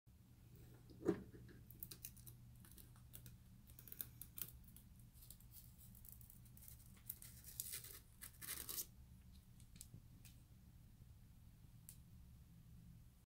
Peeling the 30 year old film off my Sega Master System
Short video of mine that's currently making the rounds on reddit, Twitter and Kotaku.